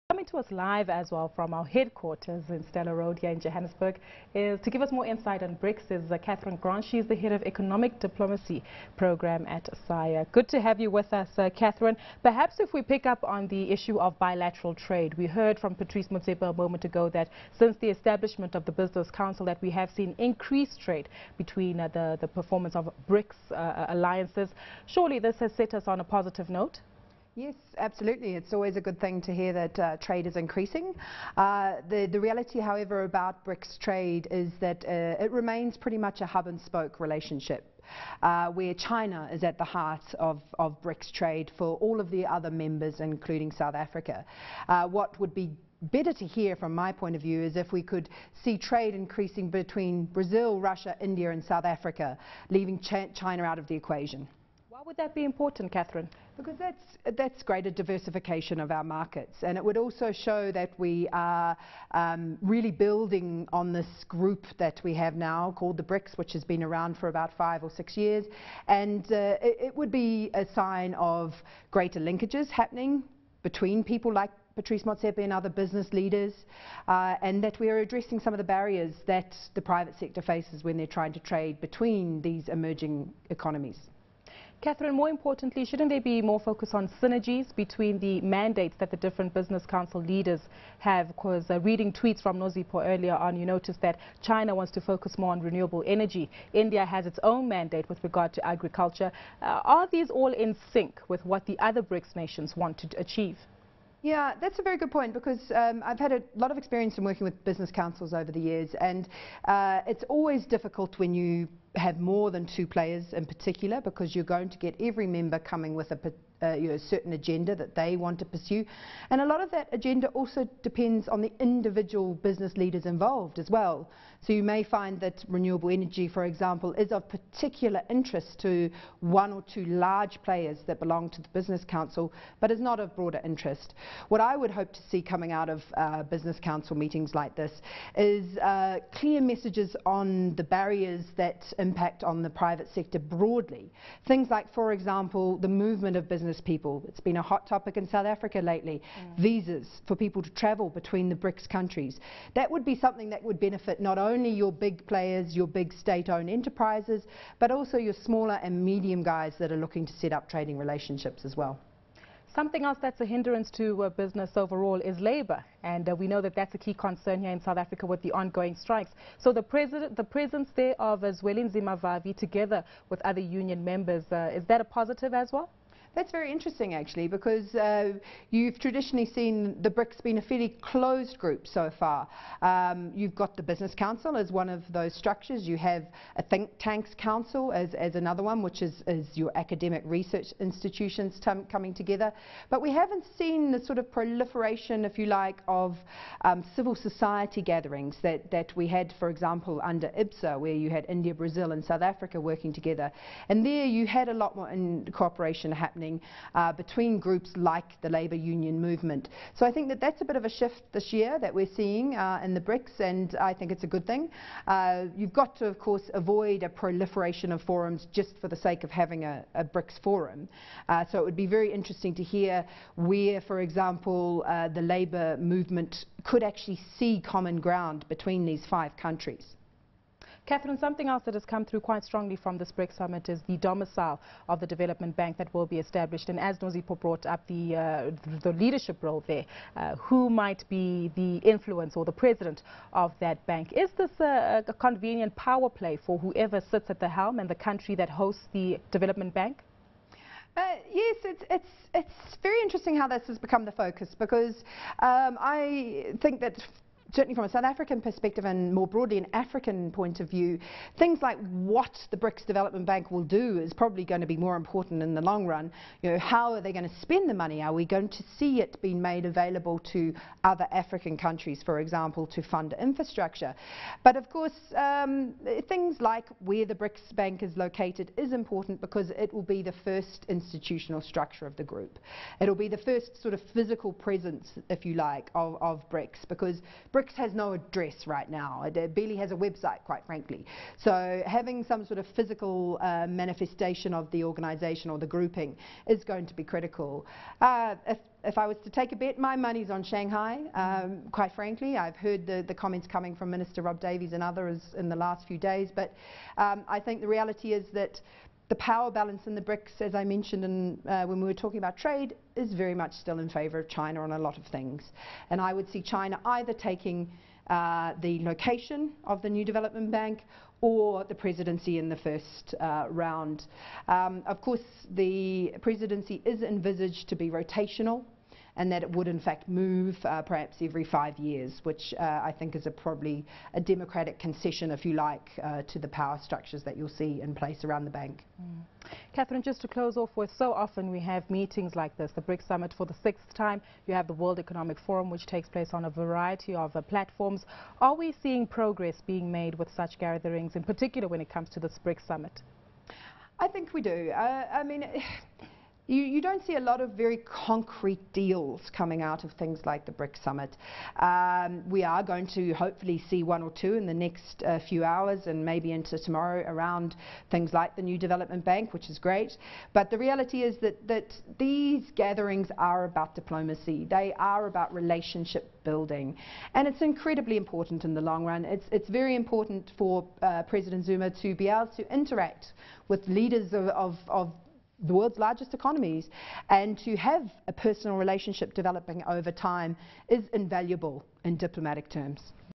Insight into BRICS summit
Original Publish Date: || Tue, 15 Jul 2014 13:36:22 GMT || Joining CNBC Africa for an insight on Brics is Catherine Grant, Head of Economic Diplomacy Programme at SAIIA.